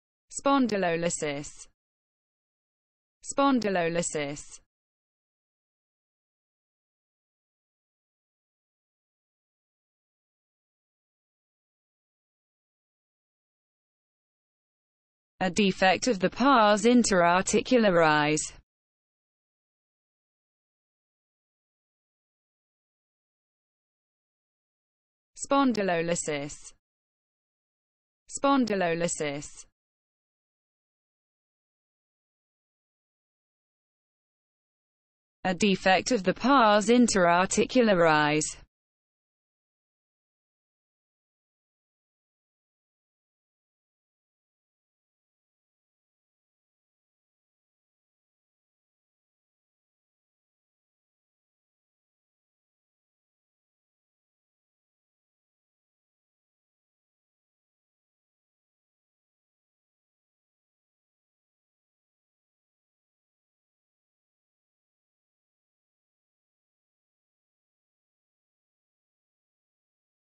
What does Spondylolysis mean?
What does Spondylolysis mean in English? Learn the meaning of the word Spondylolysis! How to Pronounce Spondylolysis. Expand your vocabulary, learn words.